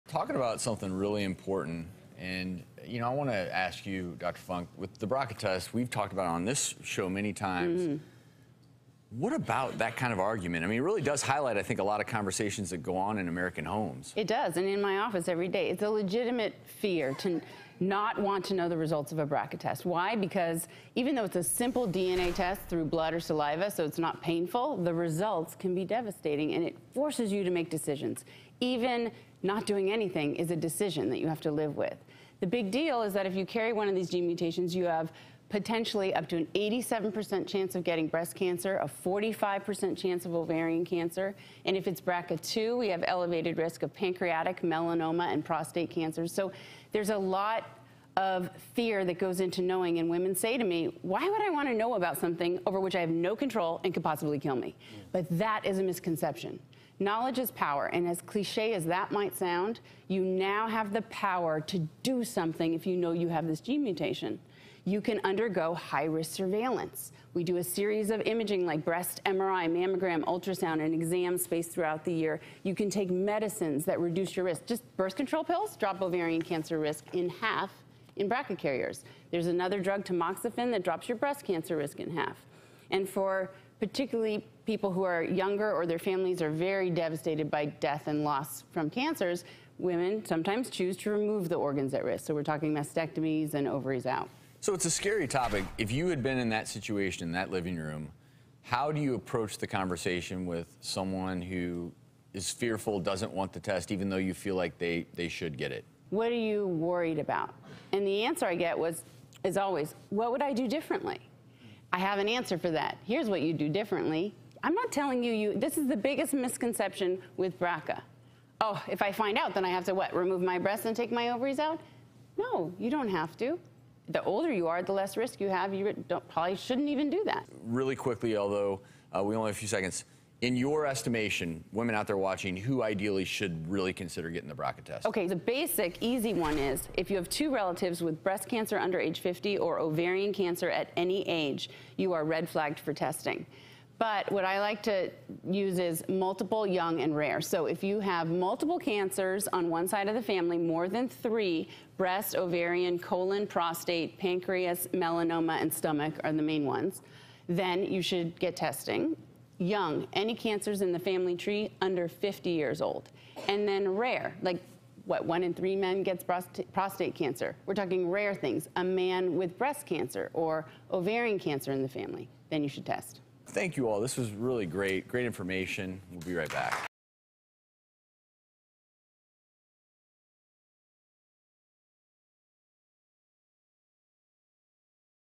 Should You Get the BRCA Gene Test?
ER physician Travis Stork is joined by surgical breast specialist Dr. Kristi Funk to discuss whether or not you should get the BRCA gene test, which can tell you if you are at risk for developing breast cancer.